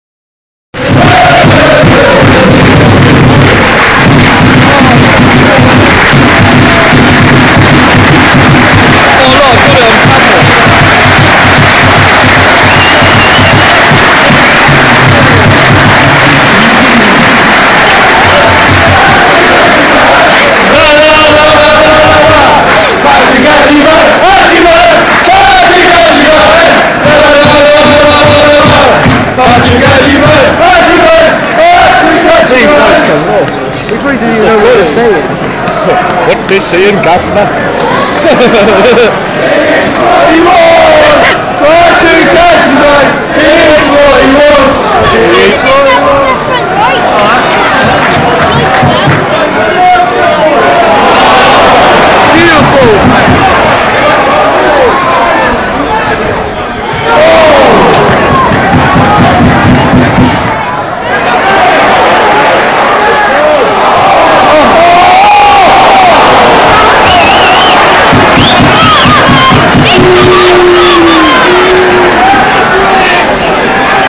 Pompey Fans v Tranmere - Patrick Agyemang Subbed Off
Pompey player, Agyemang is subbed off and Pompey fans chant banter at him. Funny. [01/04/13]